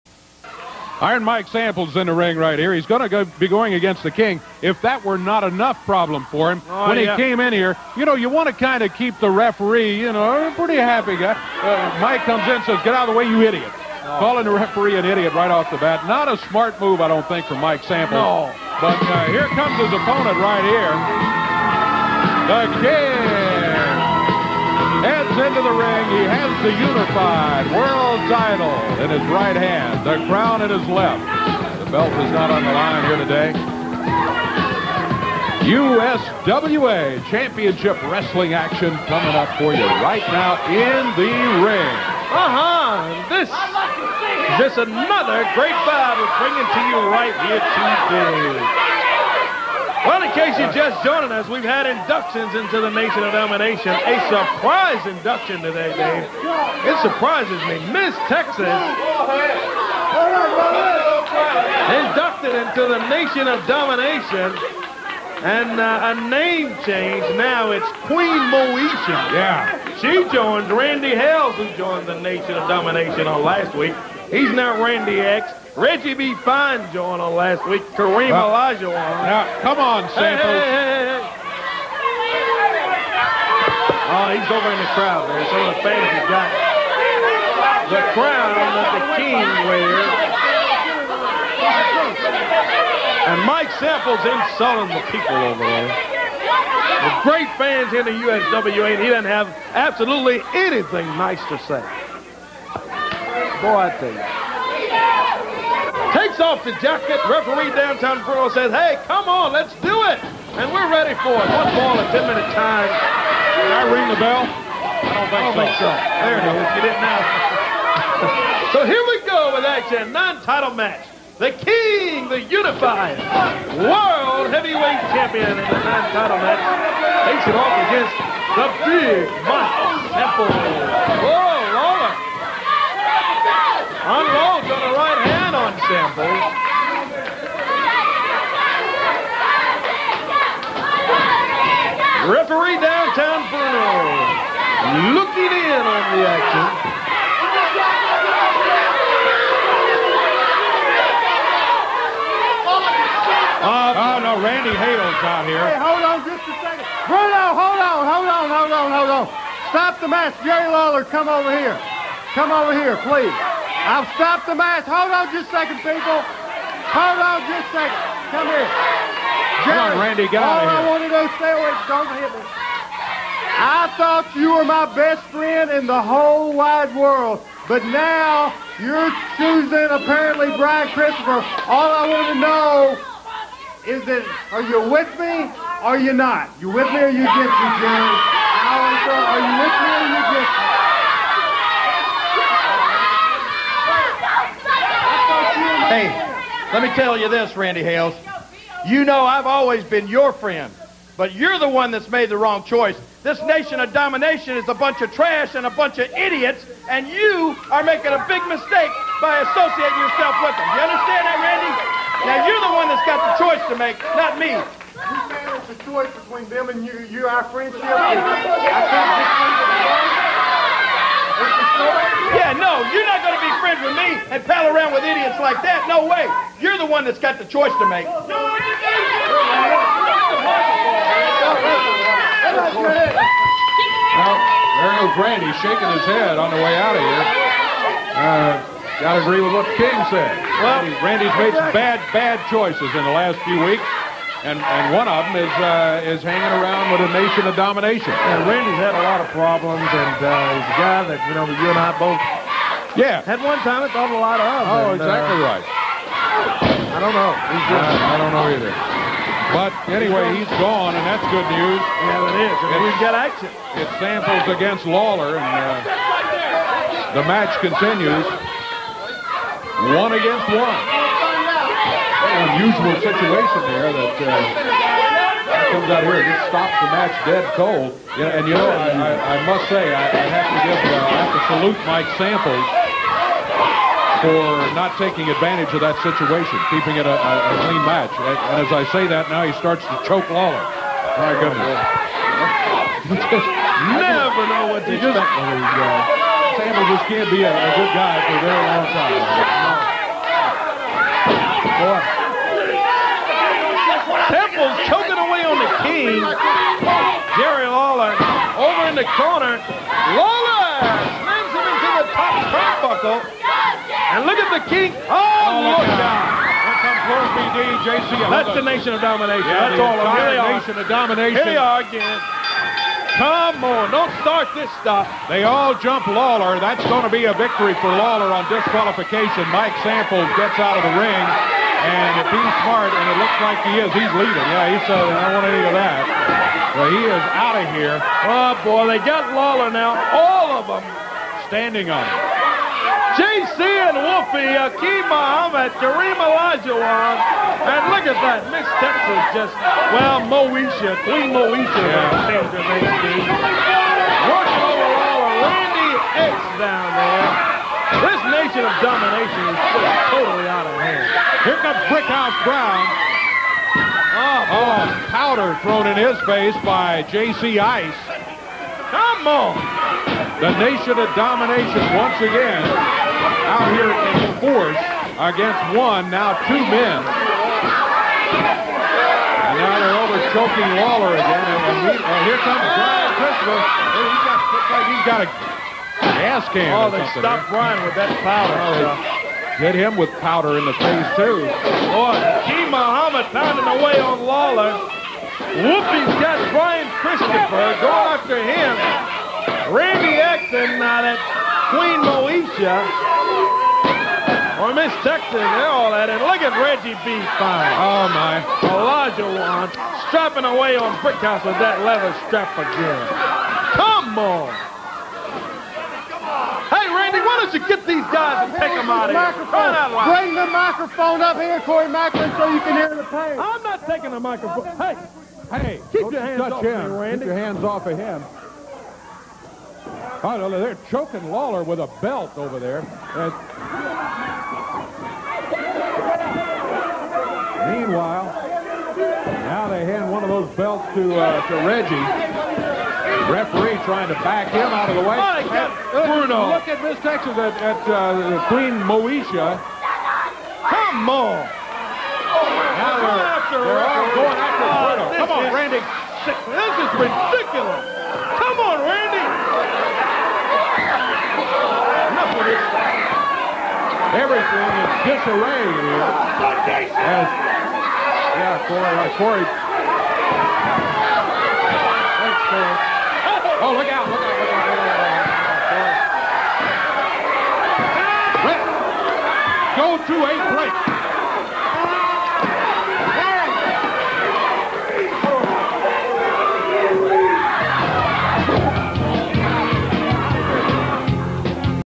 Jerry Lawler vs Mike Samples
Turns into a NOD beat down of Brian Christopher and Jerry Lawler. Even Corey Maclin gets beat up by Randy Hales and DAVE BROWN gets involved. You know shit is serious when the weather man gets involved.